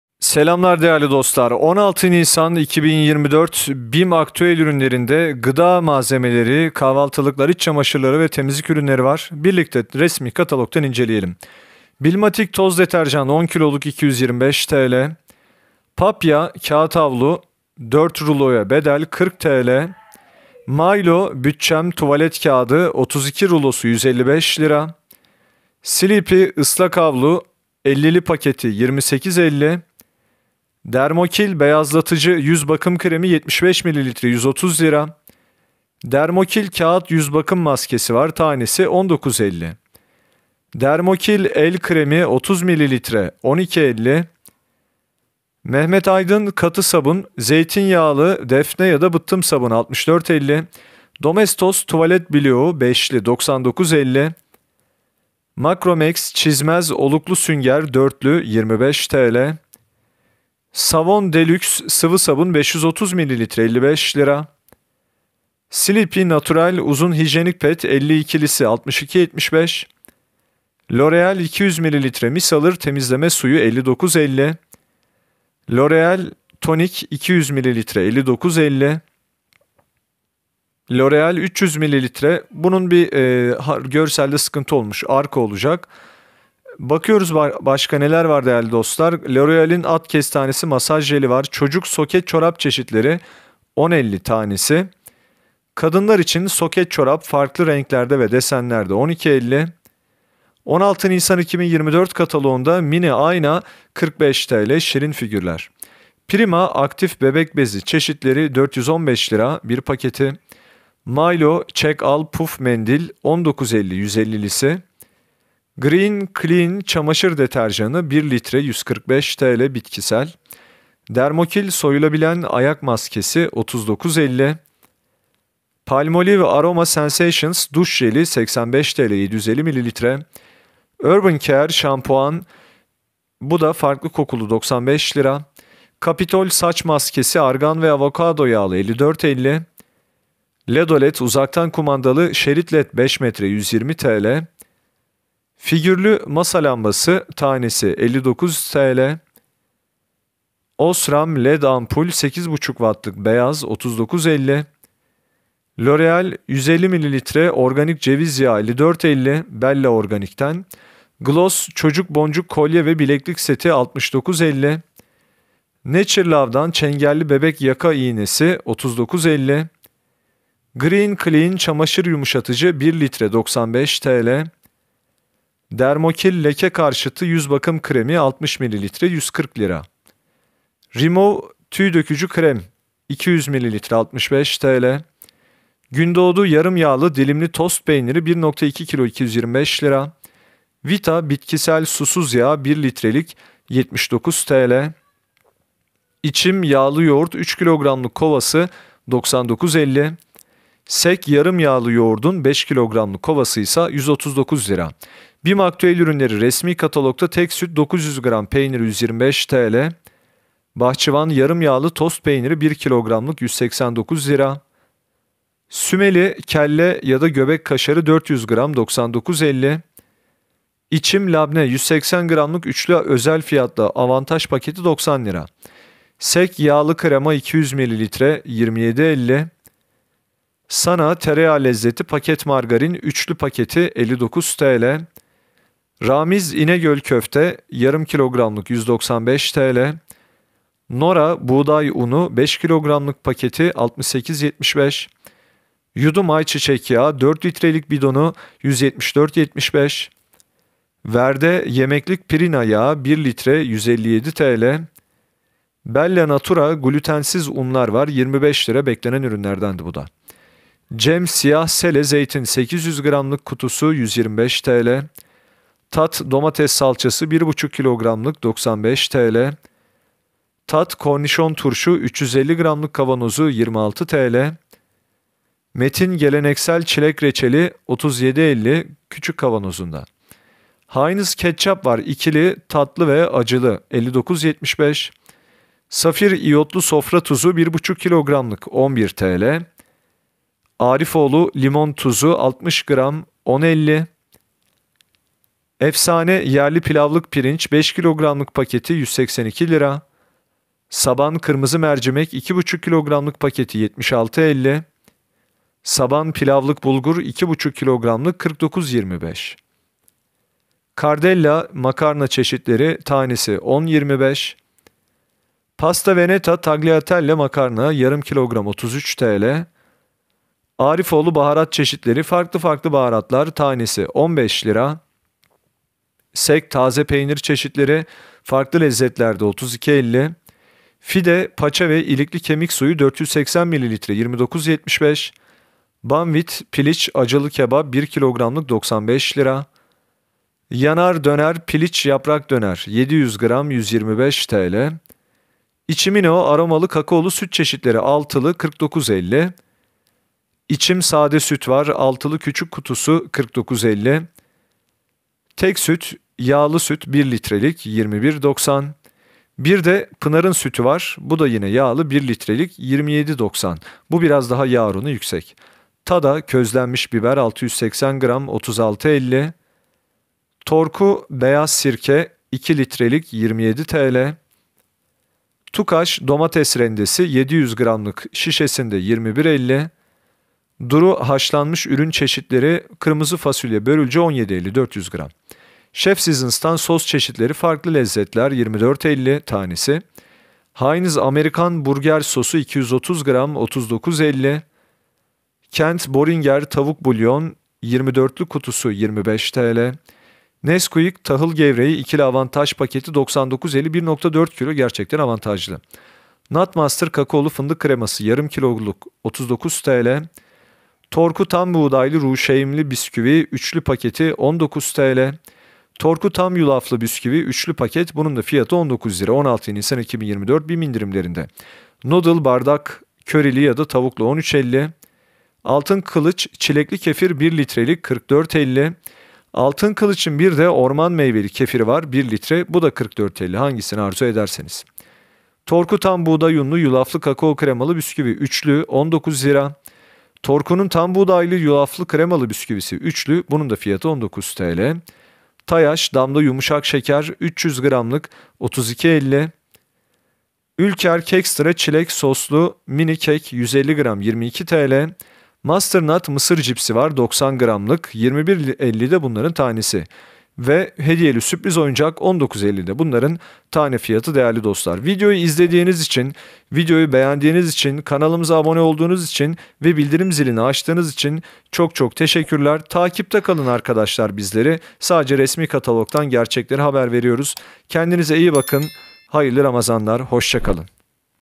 RESMİ KATALOGDAN ✅ | BİM 16 NİSAN 2024 | TEMİZLİK GIDA MALZEMELERİ | BİM AKTÜEL İNDİRİMLERİ
Bim aktüel ürünleri 16 Nisan 2024 kataloğu içinde temizlik ve gıda malzemeleri var. Bim indirimleri ve resmi katalogdan kampanyaları Ordan Burdan kanalımızda takip edebilirsiniz. Bim 23 Nisan 2024 tarihine kadar geçerli fırsatları tek net resimlerle inceleyebilirsiniz. Desteklerinize teşekkür eder saygılar sunarız.

Gündoğdu Yarım Yağlı Dilimli Tost Peyniri 1200 g 225,00
Vita Bitkisel Susuz Yağ 1 L 79,00
İçim %3 Yağlı Yoğurt 3 kg 99,50
Sek Yarım Yağlı Yoğurt 5 kg 139,00
Teksüt Tam Yağlı Beyaz Peynir 900 g 125,00
Bahçıvan Yarım Yağlı Tost Peyniri 1000 g 189,00
Sümeli Kelle&Göbek Kaşarı 400 g 99,50
İçim Labne 3x180 g 89,00
Sek Taze Peynir Çeşitleri 150 g Kekik-Zeytin Köz Acı Biberli Sade 32,50
Sek %18 Yağlı Krema 200 ml 27,50
Sana Tereyağ Lezzeti Paket Margarin 3x250 g 59,00
Fide Paça ve İlikli Kemik Suyu 480 ml 29,75
Ramiz İnegöl Köfte 500 g 195,00
Banvit Piliç Acılı Kebap 1000 g 95,00
Yanar Döner Piliç Yaprak Döner 700 g 125,00
İçimino Aromalı/Kakaolu Süt Çeşitleri  6x180 ml 49,50
İçim Süt Sade  6x200 ml 49,50
Teksüt %1 Yağlı Süt 1 L 21,90
Pınar %2,5 Yağlı Süt 1 L 27,90
Altınkılıç Çilekli Kefir 1 L 44,50
Altınkılıç Orman Meyveli Kefir 1 L 44,50
Torku Tam Buğday Unlu Yulaflı Kakao Kremalı Bisküvi 3x83 g 19,00
Torku Tam Buğdaylı Ruşeymli Bisküvi 3x80 g 19,00
Torku Tam Yulaflı Bisküvi 3x80 g 19,00
Torku Tam Buğdaylı Yulaflı Kremalı Bisküvi 3x83 g 19,00
Nora Buğday Unu 5 kg 68,75
Yudum Ayçiçek Yağı 4 L 174,75
Verde Yemeklik Pirina Yağı 1 L 157,00
Bella Natura Glutensiz Unlar Hindistan cevizi unu 300 g Keçiboynuzu unu 300 g Yulaf unu 300 g 25,00
Cem Siyah Sele Zeytin 800 g 231-290 adet/kg 125,00
Tat Domates Salçası 1500 g 95,00TL
Tada Közlenmiş Biber 680 g 36,50
Torku Beyaz Sirke 2 L 27,00
Tukaş Domates Rendesi 700 g 21,50
Tat Kornişon Turşu 350 g 26,00
Duru Haşlanmış Ürün Çeşitleri 400 g Kırmızı Fasulye Börülce 17,50
Metin Geleneksel Çilek Reçeli 37,50
Heinz Amerikan Burger Sos 230 g 39,50
Chef Seasons Sos Çeşitleri Sriracha 225 g Tatlı & Ekşi 225 g Tatlı soğan 250 g 24,50
Heinz Ketçap 2’li Set Tatlı Acı 59,75
KentBoringer Tavuk Bulyon 24x10 g 25,00
Nesquik Kakaolu Tahıl Gevreği 99,50
Safir İyotlu Sofra Tuzu 1500 g 11,00
Tayaş Damla Yumuşak Şeker 300 g 32,50
Ülker Kekstra Çilek Soslu ve Krema Dolgulu Mini Kek 150 g 22,00
Master Nut Mısır Cipsi 90 g Fıstık Ezmeli & Ballı Fıstık Ezmeli & Deniz Tuzlu 21,50
Nut Master Kakaolu Fındık Kreması 500 g 39,00
Arifoğlu Limon Tuzu 60 g10,50
Efsane Yerli Pilavlık Pirinç 5 kg 182,00
Saban Kırmızı Mercimek 2,5 kg 76,50
Saban Pilavlık Bulgur 2,5 kg 49,25
Cardella Makarna Çeşitleri Yüksük Bukle Mantı 10,25
Pasta Veneta Tagliatelle Makarna 500 g 33,00
Noodle Bardak Körili 60 g Tavuk Aromalı 60 g 13,50
Arifoğlu Baharat Çeşitleri Fesleğen 30 g Köri 50 g Zencefil 40 g Zerdeçal 40 g 11,00
Arifoğlu Baharat Çeşitleri Kajun 60 g  Toz Karabiber 40 g Kırmızı Pul Biber 70 g15, 00
Billmatik Toz Deterjan 10 kg 225,00
Papia Kağıt Havlu 2=4 Rulo 40,00
Maylo Bütçem Tuvalet Kağıdı 155,00TL
Sleepy Islak Havlu Doğal & Organik 28,50
Maylo Çek Al Puf Mendil Mandalina Kokulu 19,50
Green Clean Çamaşır Deterjanı 1 L 145,00
Green Clean Çamaşır Yumuşatıcı 1 L 95,00
Dermokil Leke Karşıtı Yüz Bakım Kremi 60 ml 139,00
Dermokil Beyazlatıcı Yüz Bakım Kremi 75 ml 129,00
Dermokil Kağıt Yüz Bakım Maskesi 19,50
Dermokil Soyulabilen Ayak Maskesi 39,50
Dermokil El Kremi 30 ml 12,50
Palmolive Aroma Sensations Duş Jeli 750 ml Feel The Massage So Relaxed Feel The Energy 85,00
Mehmet Aydın Katı Sabun Zeytinyağlı defne Bıttım 64,50
Domestos Wc Blok 99,50
Macromax Çizmez Oluklu Sünger 25,00TL
Savon De Luxe Sıvı Sabun 530 ml 55,00
Urban Care Şampuan Pink Grapefruit & Ginger 95,00
Capitol Saç Maskesi Argan Avokado 54,50
Ledolet Uzaktan Kumandalı RGB Şerit Led 5 m Kumanda (Pil Dahil) Silikonlu Şerit Led 119,00
Figürlü Masa Lambası 59,00
Osram Led Ampul 8,5 W Beyaz 39,50
Remove Tüy Dökücü Krem 2x100 ml 65,00
Sleepy Natural Uzun Hijyenik Ped 20’li + Günlük Ped 32’li Set 62,75
Loreal 200 ml Micellar Temizleme Suyu 59,50
Loreal 200 ml Tonik 59,50
Loreal 300 ml El ve Vücut Kremi Arko Badem sütlü 48,50
Loreal 150 ml Organik Hindistan Cevizi Yağı Bella Natura 54,50
Loreal 100 ml At Kestanesi Masaj Jeli Krauterhof 95,00
Çocuk Soket Çorap Çeşitleri 10,50
Kadın Soket Çorap 12,50
Gloss Çocuk Boncuk Kolye ve Bileklik Seti 69,50
Naturalove Çengelli Bebek Yaka İğnesi 39,50
Mini Ayna 45,00
Cosby Süpriz Hediyeli Oyuncak 19,50
Prima Aktif Bebek Bezi Çeşitleri 415,00